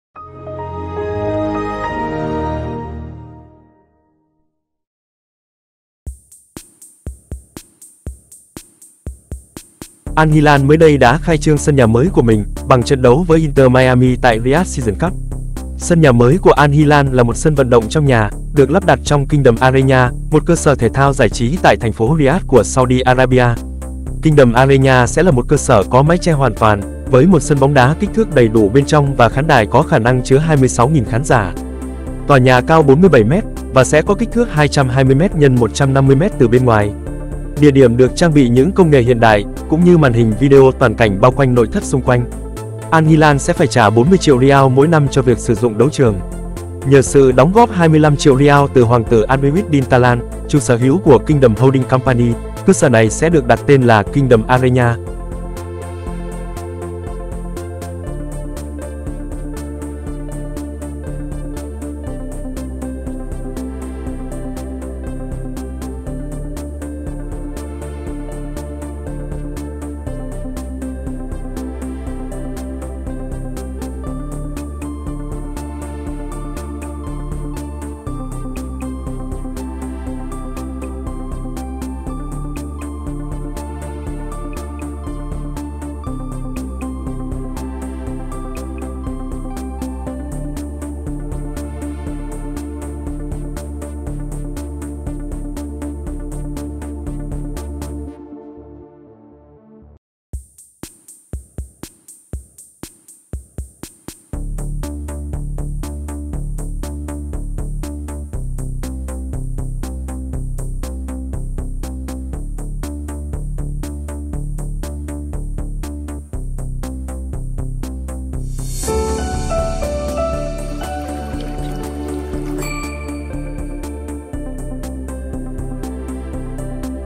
AL-HILAL TRÌNH LÀNG SVĐ TRONG NHÀ Ở TRẬN ĐẤU VỚI ĐỘI BÓNG CỦA MESSI
►Ủng hộ cho team:
►Nguồn/ Special thanks to: